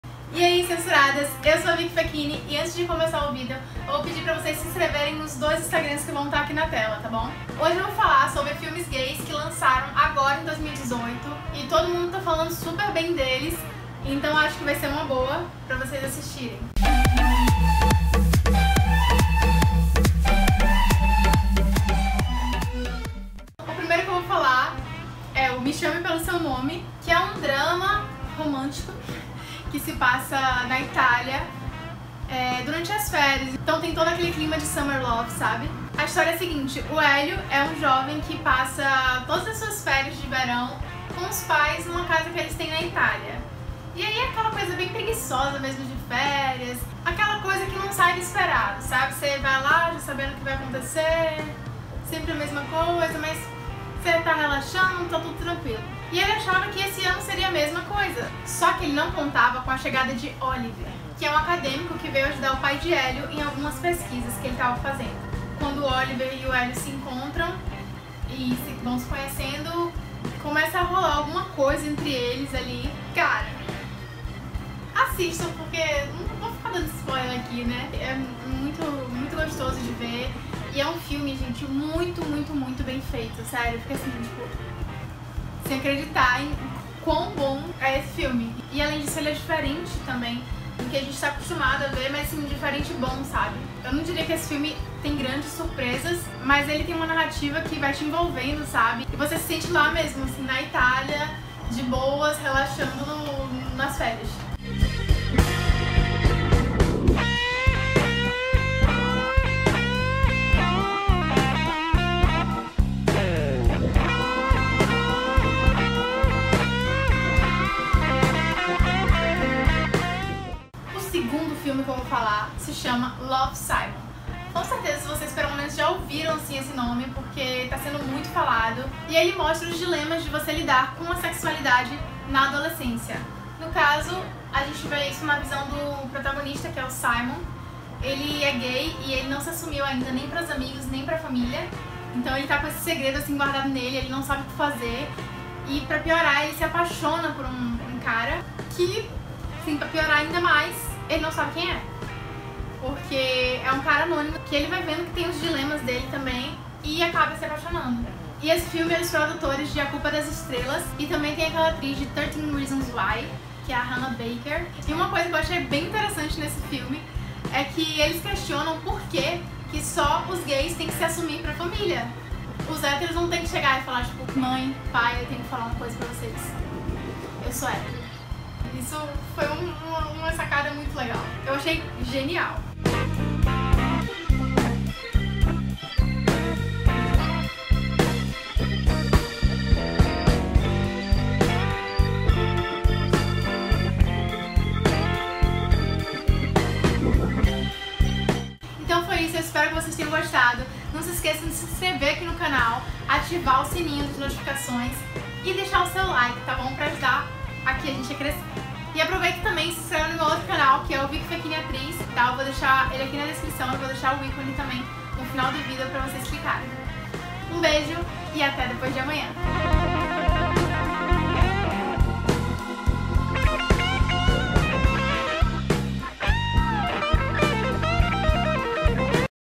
Filmes Gays de 2018
Hoje eu vou indicar algumas estreias gays do cinema, que estão super bem na fita para vocês assistirem e se apaixonarem.

Canal: @canalcensuradas
Vicky: @vickyfechine

1- Me chame pelo seu nome
2- Love, Simon